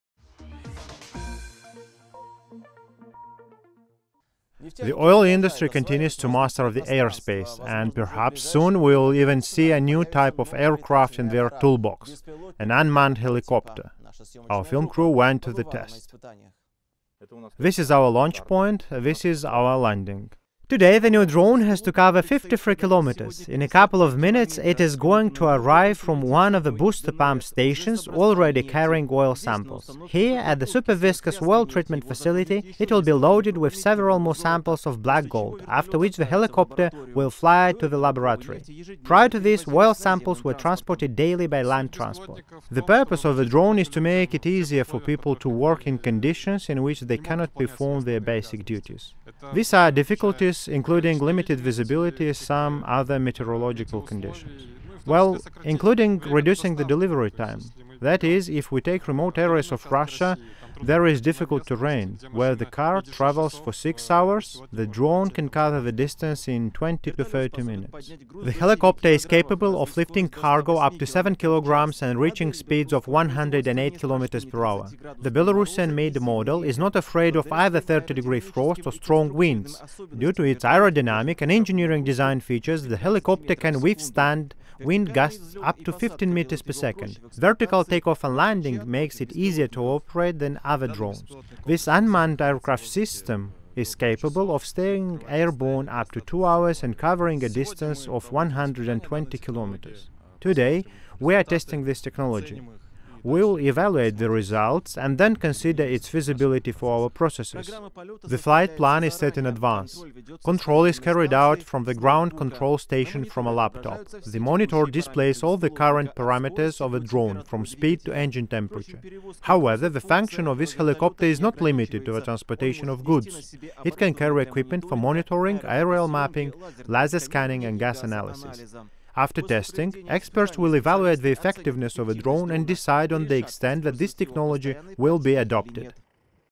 New application of drones in Oil&Gas
Electric unmanned helicopter aOrion Electro performed flights for Tatneft, the Oil&Gas company.

The drone carried out video monitoring of pipelines. It also perform an untrivial task for a small unmanned aircraft: transported oil samples, a 6kg payload, from a booster pump station to the laboratory. The drone's high capacity made these tasks possible at critical temperature up to -30°C and strong wind up to 12 m/s.